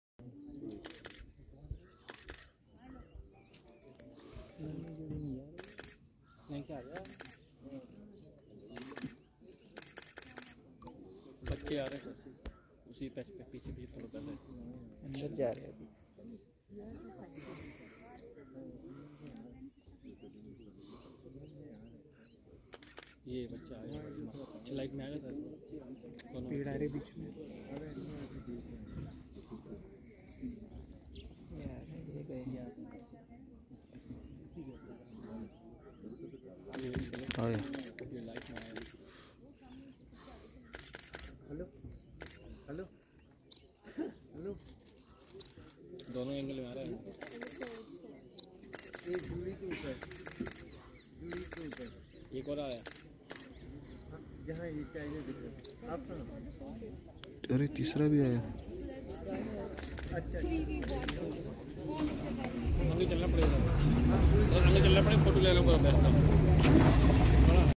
Tigress T 151 with her two subadult cubs at Panna Tiger Reserve
Video is Recorded By
                                    : Avinash Mondhe
                          Gate  : Hinouta Gate, Panna Tiger Reserve

    Technical Support : Amit Thute
                                       Ajinkya Hanumante